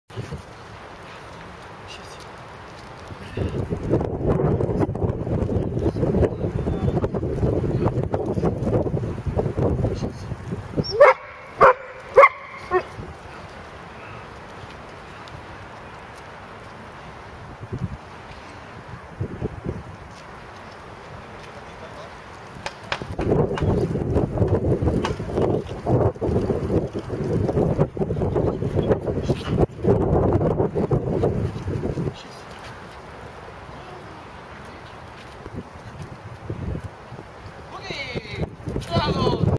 Iuca si colegii de dresaj - Ralph (2)
Ore de dresaj canin cu Georgian Negrescu